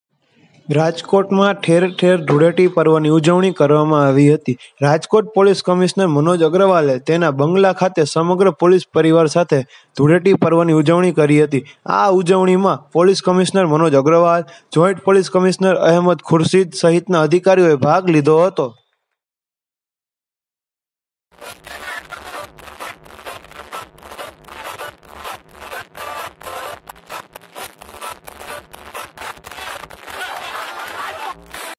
રાજકોટ: પોલીસ કમિશ્નર મનોજ અગ્રવાલ સહીતનાઓએ ધૂળેટીની ઉજવણી કરી
રાજકોટ, તા.11
રાજકોટમાં ઠેર-ઠેર ધૂળેટી પર્વની ઉજવણી કરવામાં આવી હતી. રાજકોટ પોલીસ કમિશનર મનોજ અગ્રવાલે તેના બંગલા ખાતે સમગ્ર પોલીસ પરિવાર સાથે ધૂળેટી પર્વની ઉજવણી કરી હતી. આ ઉજવણીમાં પોલીસ કમિશનર મનોજ અગ્રવાલ, જોઇન્ટ પોલીસ કમિશનર અહેમદ ખુરશીદ સહિતના અધિકારીઓએ ભાગ લીધો હતો.
(અહેવાલ: સંદિપ કાલાવડીયા)